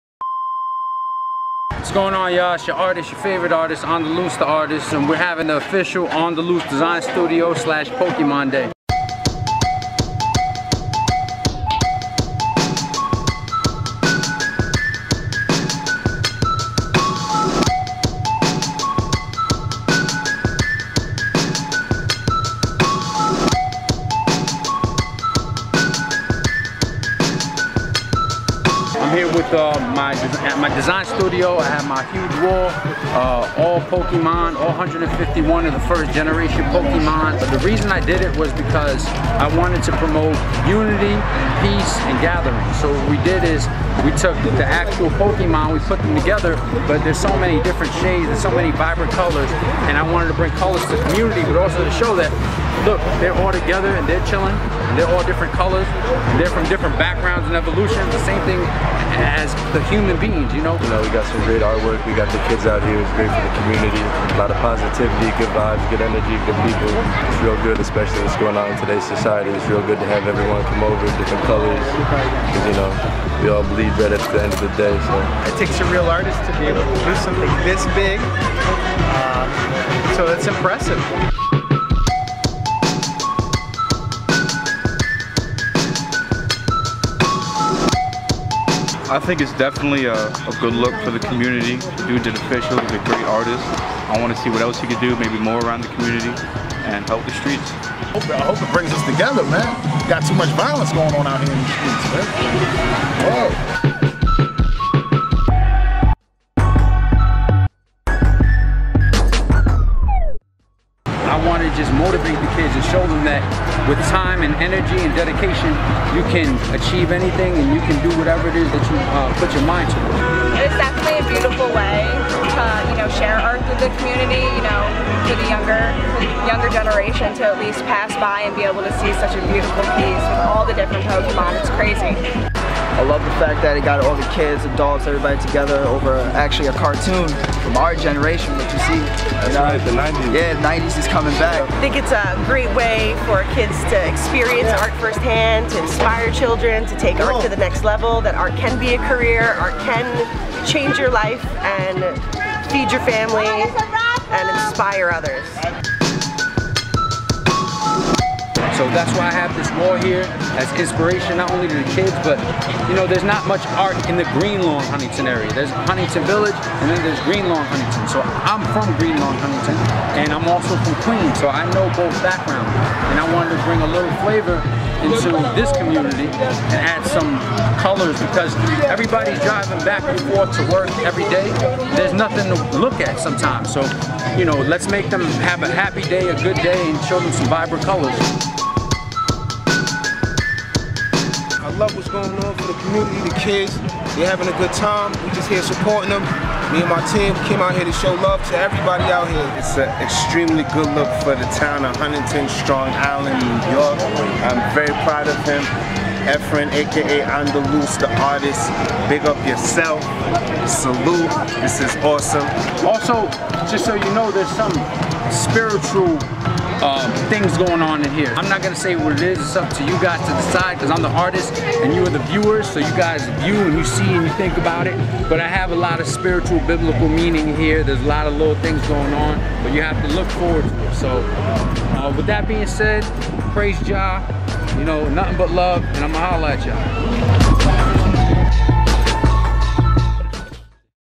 TkActnTV: Andaluz The Artist - Creating the PokeWall
Check out as @AndaluzTheArtist creates the widely popular "PokeWall" in Huntington, NY!